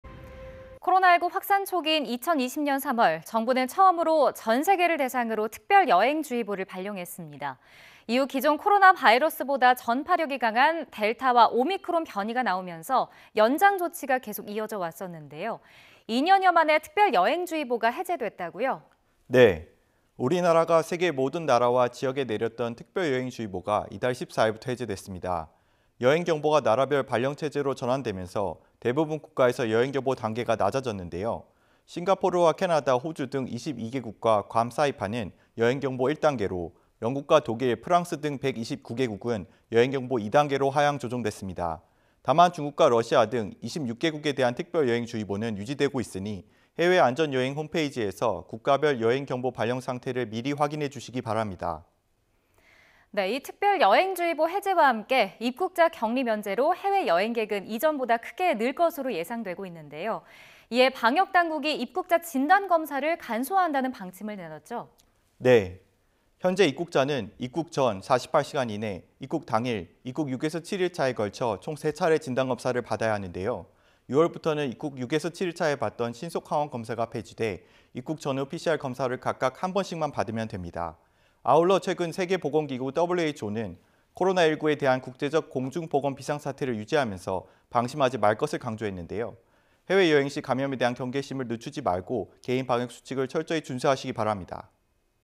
전 세계 특별여행주의보 해제 [해외안전여행정보] / YTN korean
코로나19 확산 초기인 2020년 3월, 정부는 처음으로 전 세계를 대상으로 특별여행주의보를 발령했습니다.이후 기존 코로나 바이러스보다 전파력이 강한 델타와 오미크론 변이가 나오면서 연장 조치가 계속 이어져 왔었는데요.2년여 만에 특별 여행주의보가 해제됐다고요?

[사무관]
네, 우리나라가 세계 모든 나라와·지역에 내렸던 특별여행주의보가 이달 14일부터 해제됐습니다.

여행경보가 나라별 발령 체제로 전환되면서, 대부분 국가에서 여행경보 단계가 낮아졌는데요.

싱가포르와 캐나다, 호주 등 22개국과 괌, 사이판은 여행경보 1단계로, 영국과 독일, 프랑스 등 129개국은 여행경보 2단계로 하향 조정됐습니다.

다만 중국과 러시아 등 26개국에 대한 특별여행주의보는 유지되고 있으니 해외안전여행 홈페이지에서 국가별 여행경보발령 상태를 미리 확인해주시기 바랍니다.

[앵커]
특별여행주의보 해제와 함께 입국자 격리 면제로 해외 여행객은 이전보다 크게 늘 것으로 예상되고 있는데요.

이에 방역 당국이 입국자 진단검사를 간소화한다는 방침을 내놨죠?

[사무관]
네, 현재 입국자는 입국 전 48시간 이내 입국 당일, 입국 6∼7일 차에 걸쳐 총 세 차례 진단검사를 받아야 하는데요.

6월부터는 입국 6∼7일 차에 받던 신속항원검사가 폐지돼, 입국 전후 PCR 검사를 각각 한 번씩만 받으면 됩니다.

아울러 최근 세계보건기구(WHO)는 코로나19에 대한 국제적 공중보건 비상사태를 유지하면서 방심하지 말 것을 강조했는데요.

해외여행 시 감염에 대한 경계심을 늦추지 말고 개인 방역 수칙을 철저히 준수하시기 바랍니다.